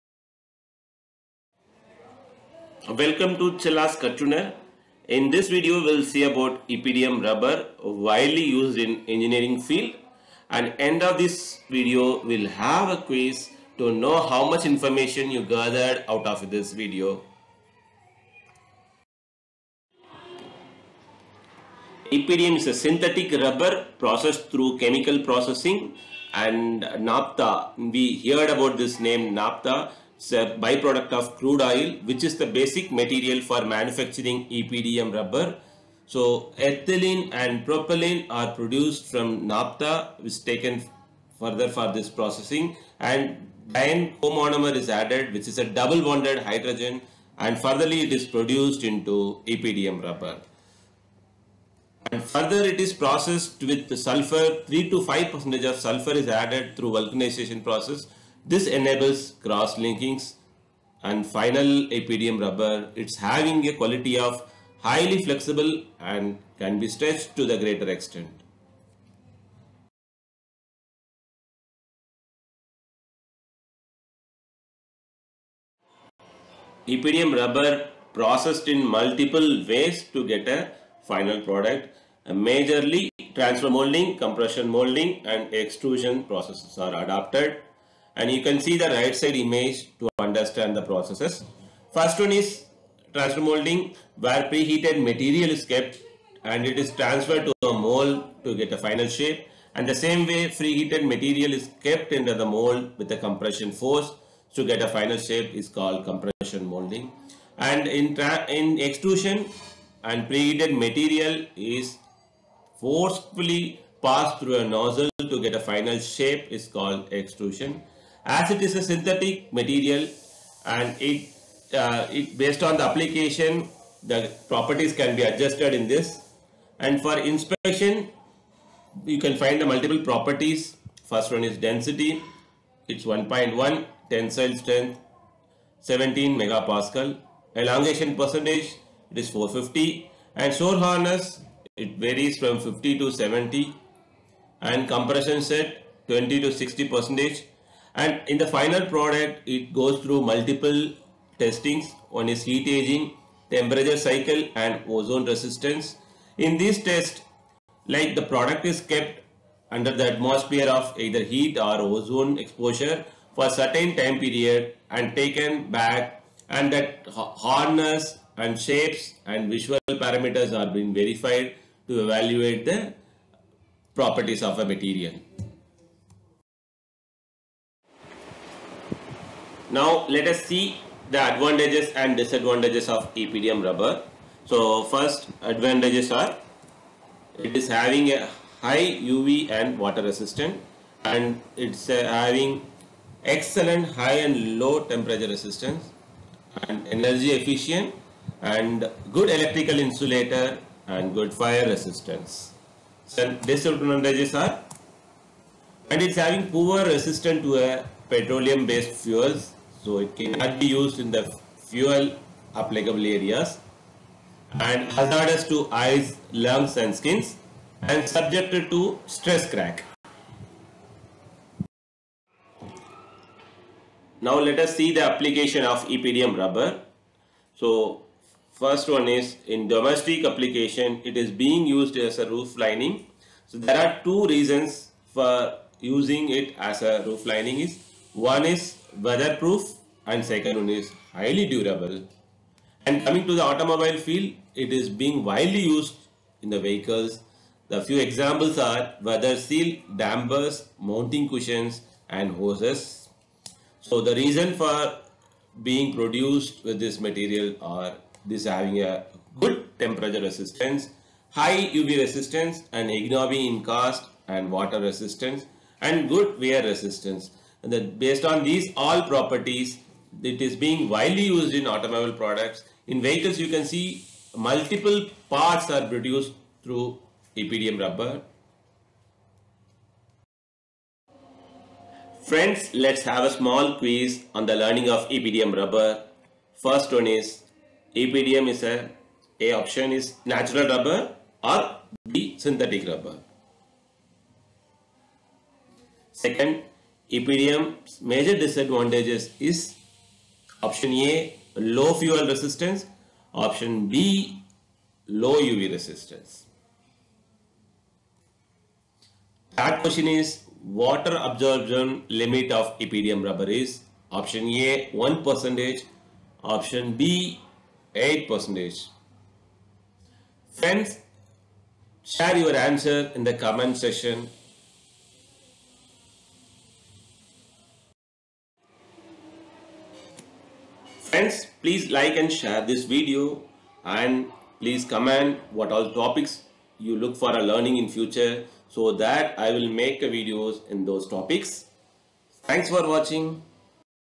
EPDM Rubber Introduction ( Engineering Application )
This video explains  the following content on EPDM rubber

1. Introduction
2. Process and Inspection
3. Advantages & Disadvantages
4. Application

Majorly covers the engineering application